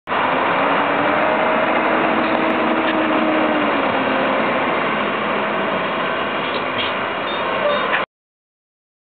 Trackless Tack 2
Trackless Tack application on surface bus lot on Stelzer Rd. Columbus, OH. May 6, 2009.